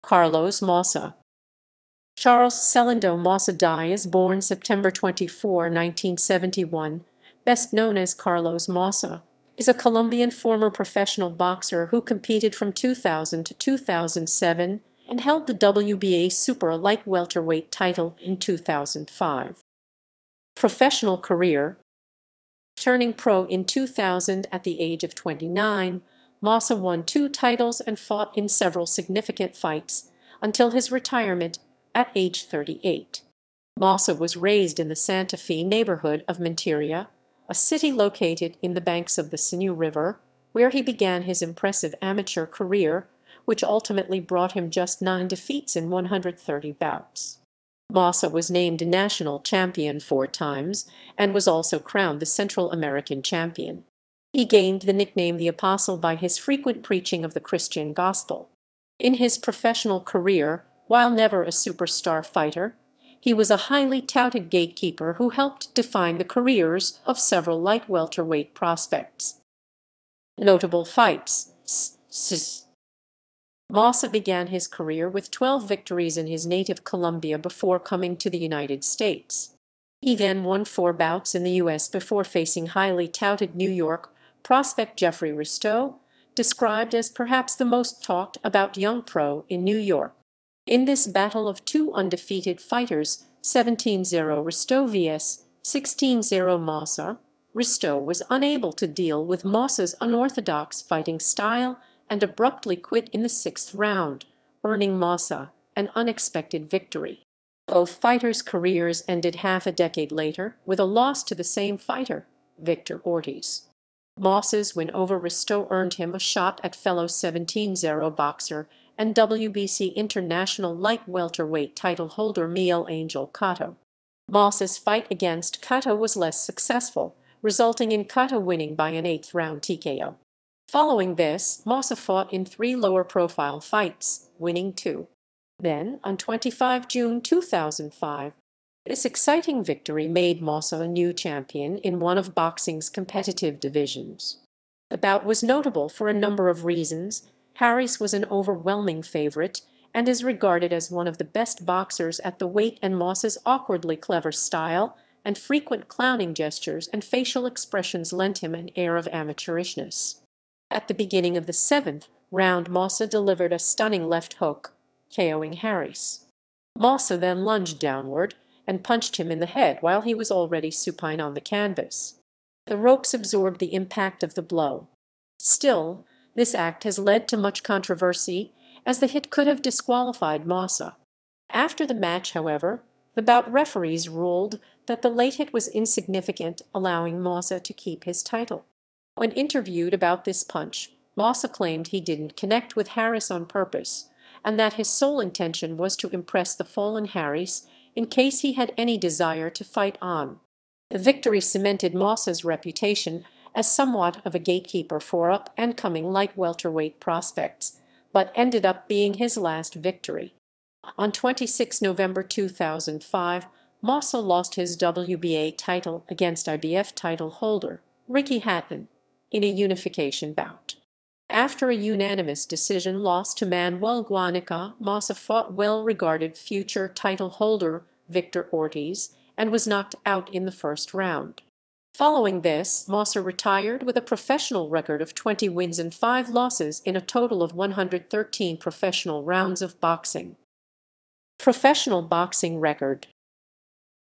Carlos Maussa
Charles Celindo Maussa Diaz (born September 24, 1971), best known as Carlos Maussa, is a Colombian former professional boxer who competed from 2000 to 2007, and held the WBA super light welterweight title in 2005.